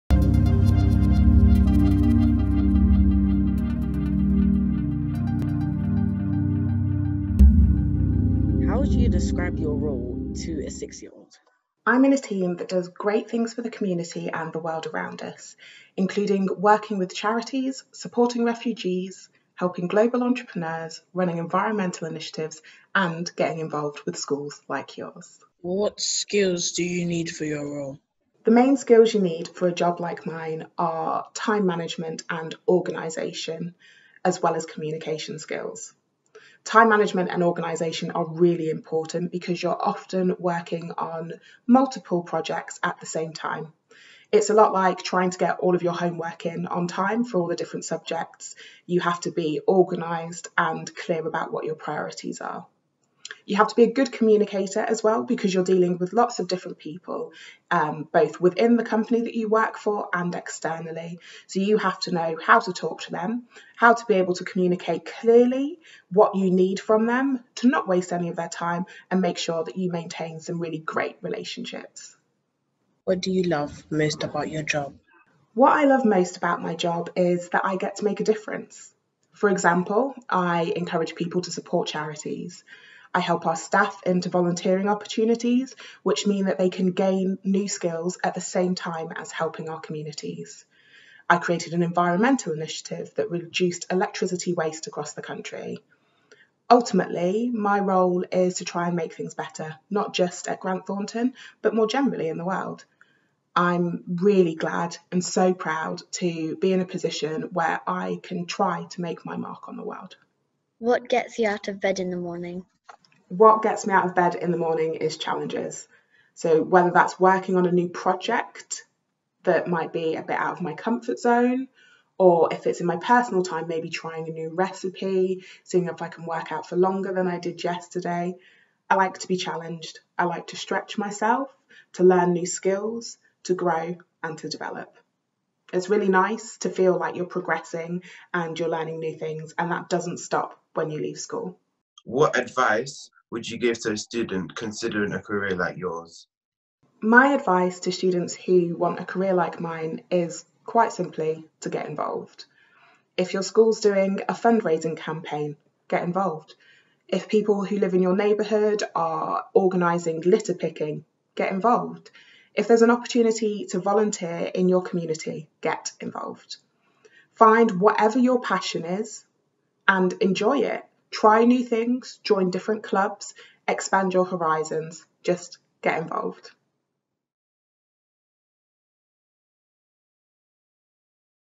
Talking careers – Fayola
Fayola Francis, Sustainability Executive at Grant Thornton, talks about her career and shares advice to help students to unlock their potential.